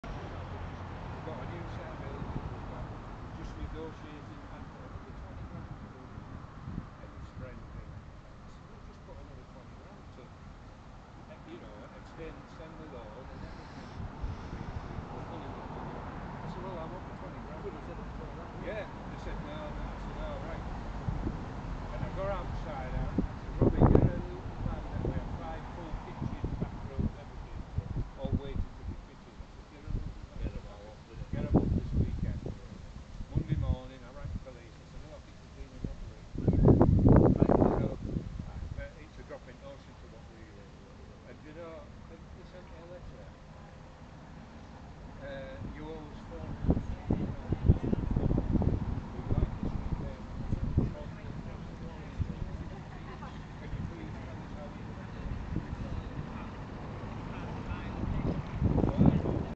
special boat bonanza 2023
here onboard the cruiser boat we again see the ripon sceanic cruises trip boat passing the cruiser boat heading to pick more passengers up